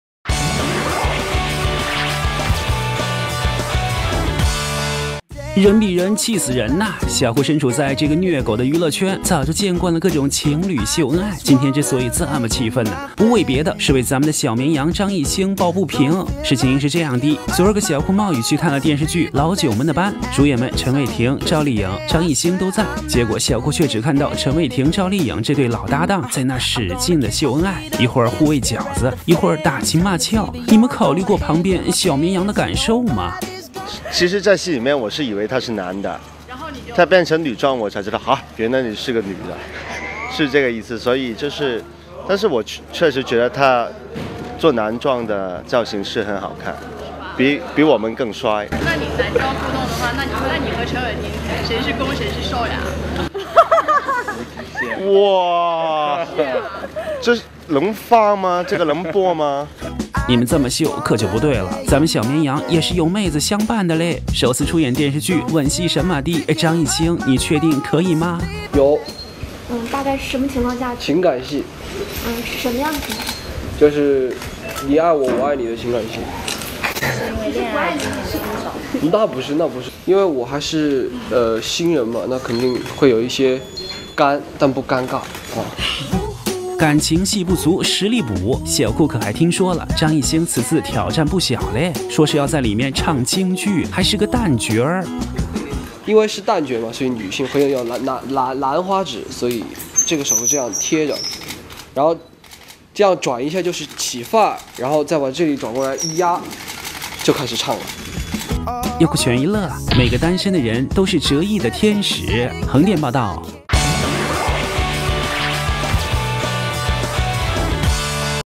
151223 [youku] Zhang Yixing Lay @ Old Nine Gates 老九门 filming
张艺兴自曝感情戏不尴尬 陈伟霆赵丽颖再搭档火花四溅 151223